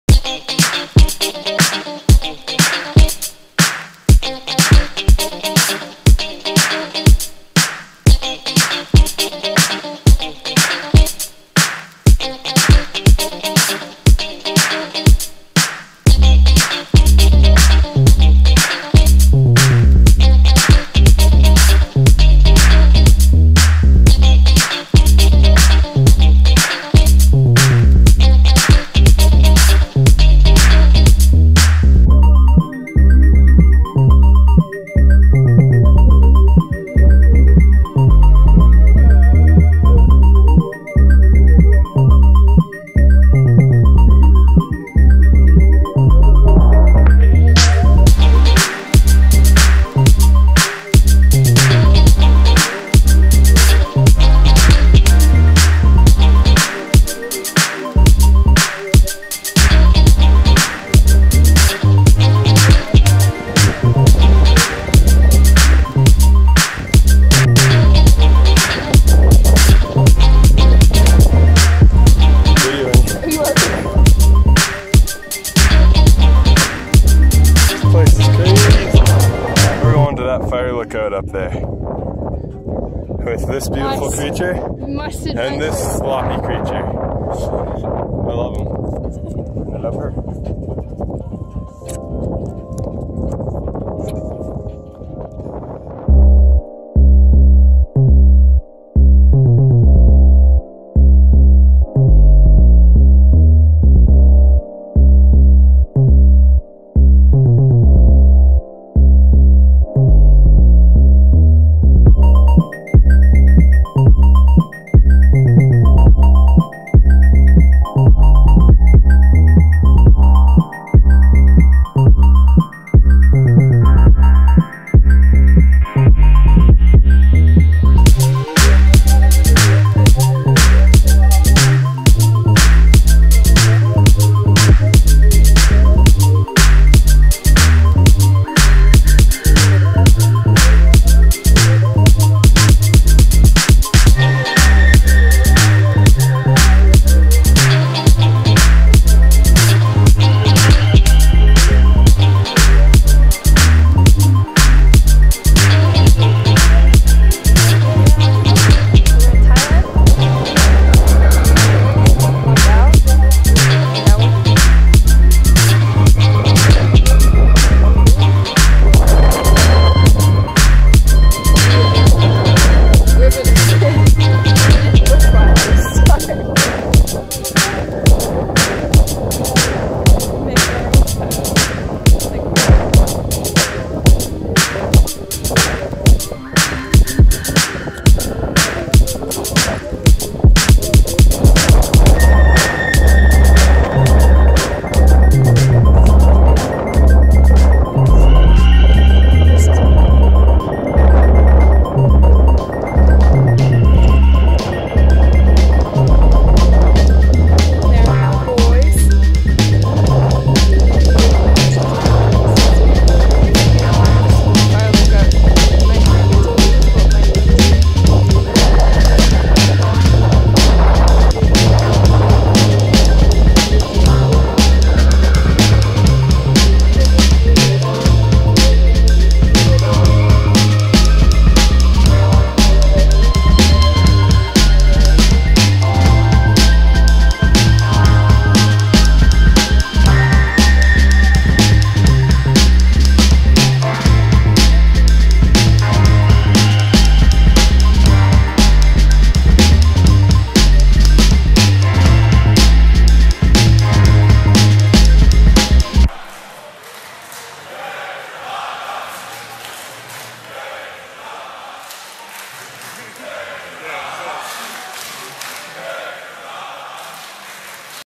Mount Burke Hike [Alberta, Canada]
(July, 2018) This hike is located in the Highwood Region and is most easily accessed via highway 541 from Longview, Alberta. There is an old, slightly decrepit fire-lookout hut at the summit which is an awesome lil' piece of history to explore. This was Kitzbühel Burk's first hike! (Meaning it is very dog friendly!)